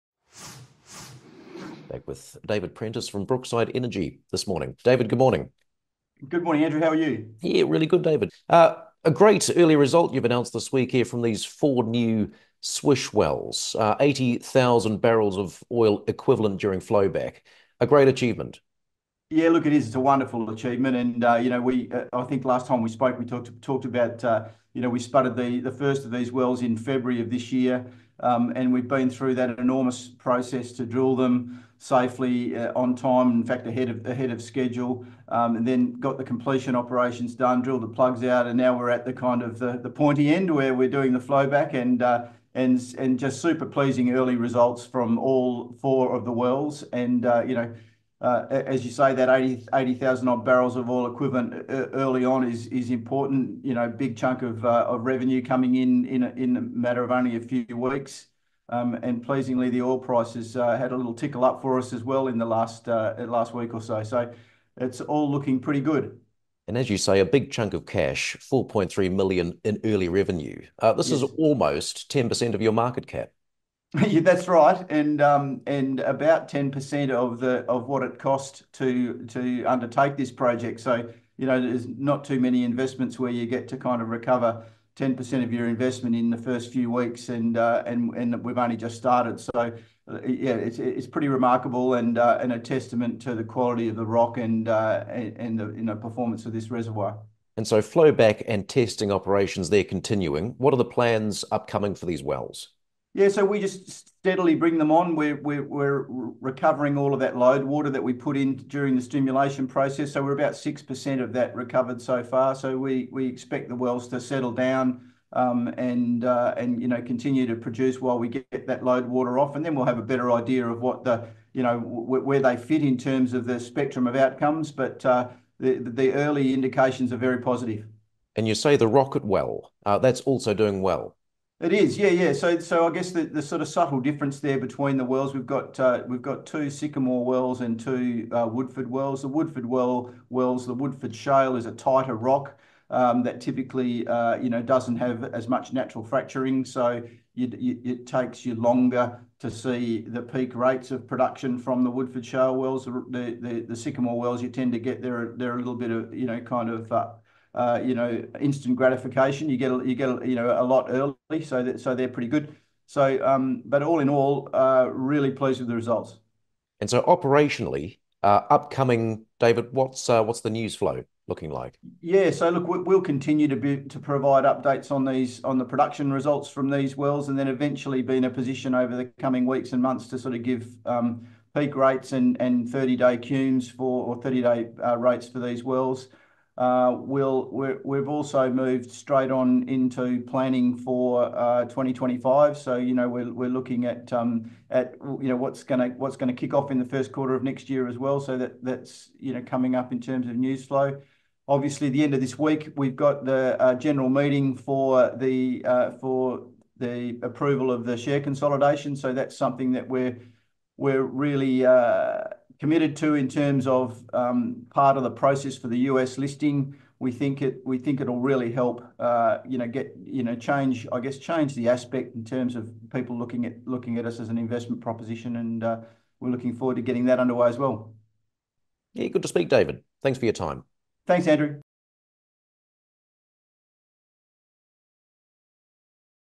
Brookside Energy Tells JustStocks Video About Early Revenue From New SWISH Wells In Oklahoma
Brookside Energy Ltd (ASX:BRK) MD David Prentice speaks with JustStocks Video after announcing the company has banked additional revenues of around US$4.3 million since flowback and testing began last month on its four new FMDP wells at the SWISH Project in Oklahoma’s Anadarko Basin.

Highlights

Four new Flames Maroons Development Plan (FMDP) wells at the Company’s flagship SWISH Project in Oklahoma, produce approximately 80,000 barrels of oil equivalent (BOE) (87% liquids). in the first weeks of flowback and testing.
Significant early gross revenue of approximately US$4.3 million generated with more sales ahead as flowback and testing operations continue.
Combined daily production reaches approximately 3,900 barrels of oil equivalent per day (BOEPD) with less than 6% of stimulation fluid recovered.
IP24 (peak rate), IP30, and IP90 rates will be reported for each well as these milestones are achieved.

The three Sanford Pad wells (Fleury, Maroons, and Iginla) are producing significant volumes of oil and gas, notwithstanding ~75% of the total completed lateral length of these wells is within the Woodford Shale, a formation that typically takes longer to clean up and begin producing compared to the highly fractured Sycamore Limestone.

Production from the Rocket Well (two-mile lateral in the Sycamore formation) is also showing strong early volumes during flowback and testing.

Approximately 6% of the total fluid used during the stimulation of these wells has been recovered so far in this early phase of flowback and testing.

The FMDP

The FMDP is a multi-well drilling program targeting the highly productive Sycamore Lime and Woodford Shale formations in the SCOOP area of the southern Anadarko Basin.

Of the four wells in the multi-well drilling program, three were drilled from the Sanford Pad; the Fleury, Maroons, and Iginla Wells, with the final well, the Rocket Well, was drilled from the Flames Well pad